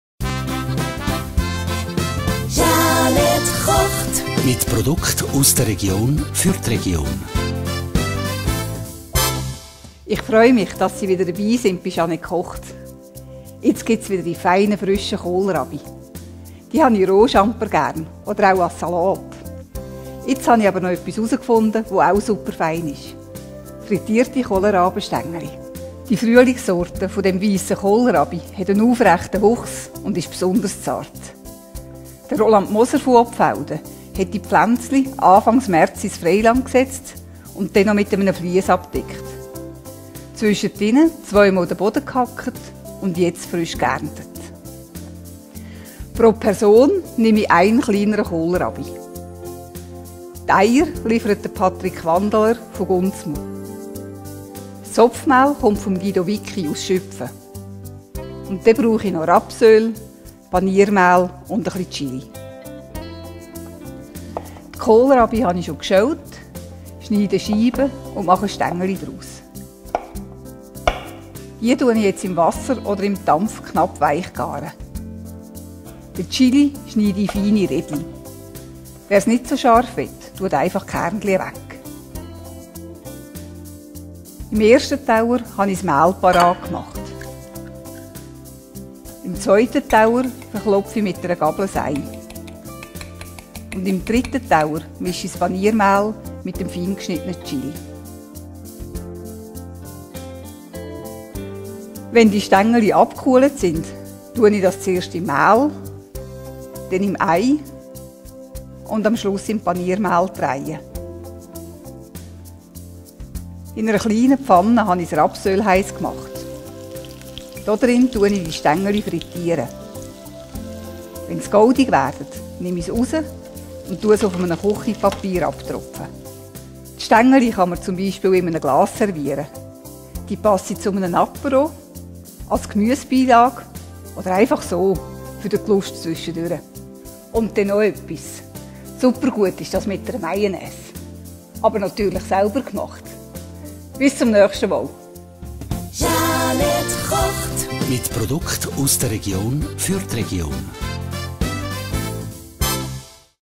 Kohlrabi-Stengeli
Kohlrabi schälen und in Stängeli schneiden. Chili in kleine Stücke schneiden. In je einem Teller die geschlagenen Eier, das Mehl sowie das Paniermehl mit dem geschnittenen Chili vorbereiten.

Stängeli im Wasser oder im Dampf knapp weich garen. Abgekühlte Stängeli zuerst im Mehl, dann im geschlagenen Ei und am Schluss im Paniermehl wenden. Im heissen Rapsöl frittieren.